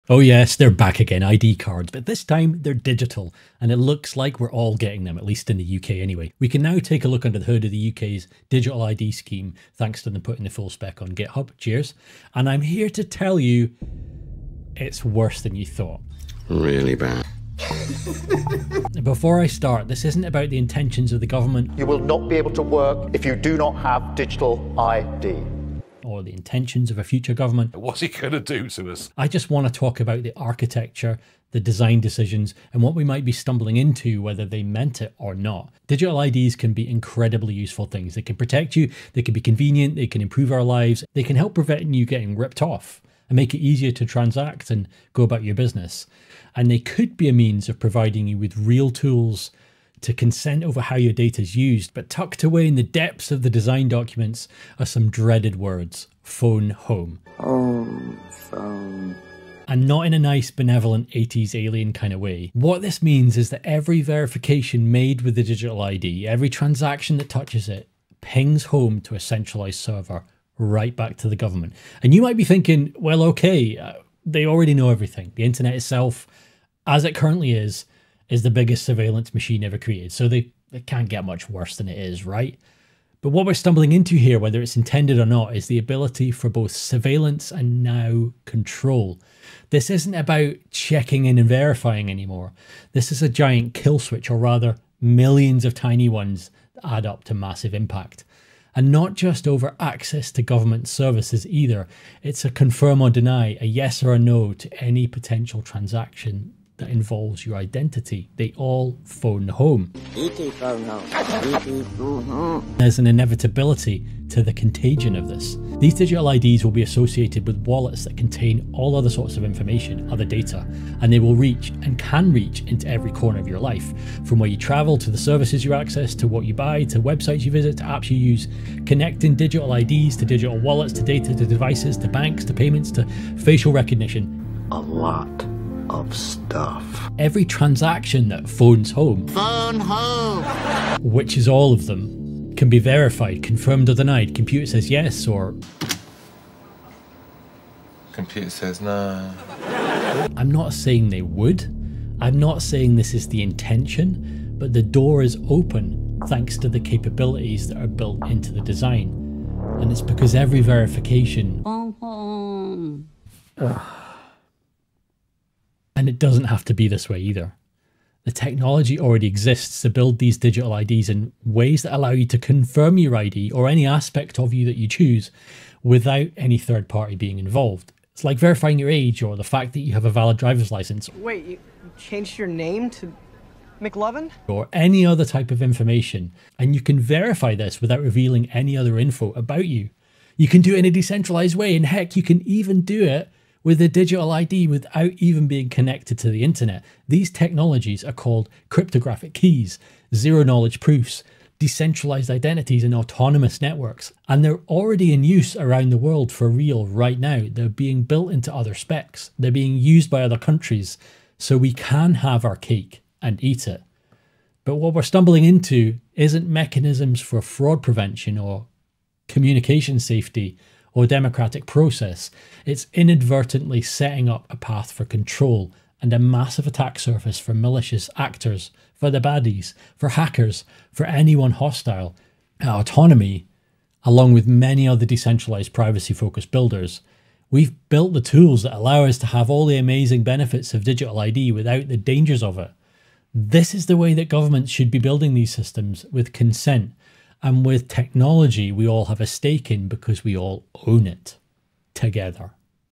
UK Digital IDs Are Worse Than You Thought
The UK government has put their digital ID specs on GitHub. We took a look under the hood and it's worse than you could imagine.

But there's a better way! Using cryptographic keys, zero-knowledge proofs, and decentralised networks. Technology that already exists. Technology we're building.

The alternative future we're building: 🌐 Autonomi - A new internet owned by everyone, controlled by no one